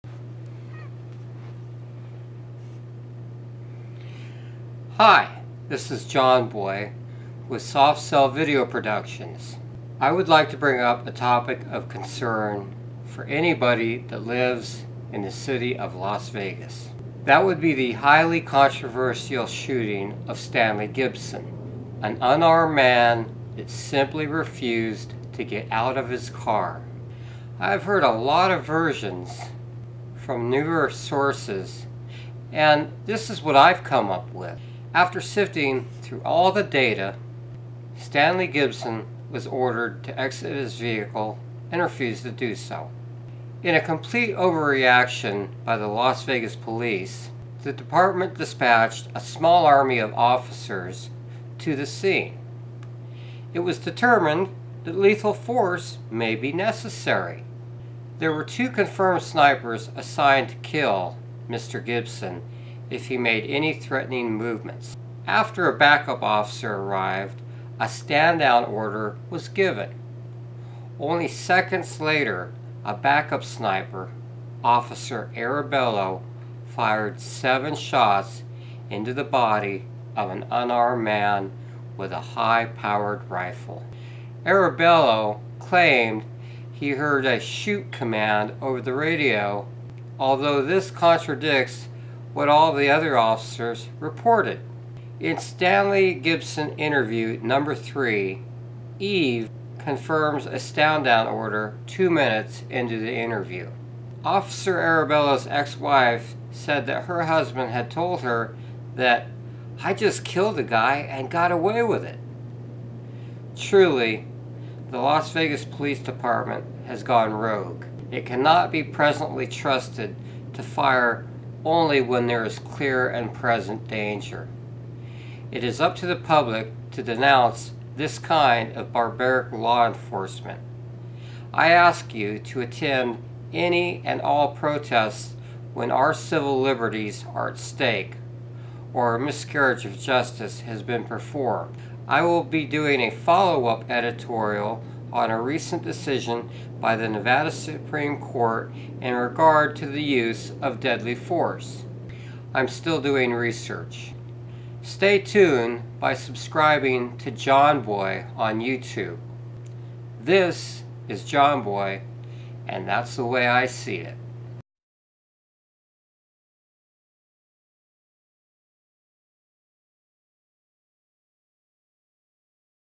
Stanley Gibson, a senseless death
This is an editorial by independent journalist Jawn Bouy. It highlights the lack of supervision of Las Vegas Police in matters of Life and Death situations.